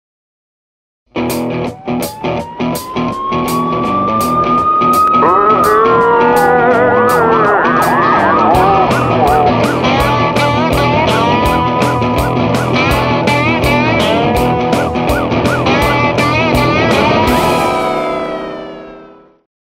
4. American Film Festival - zwiastun
UNITED STATES OF CINEMA/ WSZYSTKIE STANY KINA. American Film Festival in Wrocław, Poland October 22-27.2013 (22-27 pażdziernika)